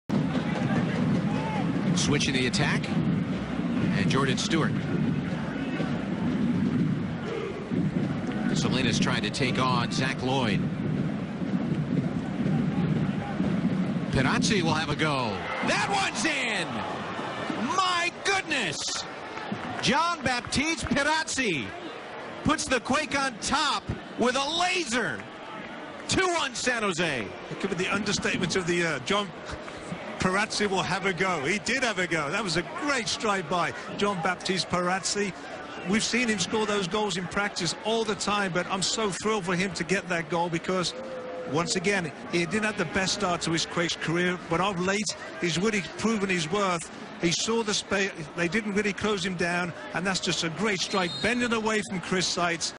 Pierazzi JB GOAL magical strike from distance FC Dallas vs San Jose Earthquakes1
Le plus beau but de l'année aux USA est signé Jean-Baptiste PIERAZZI d'une frappe surpuissante de 40 métres (chronofoot.fr 1.06.14)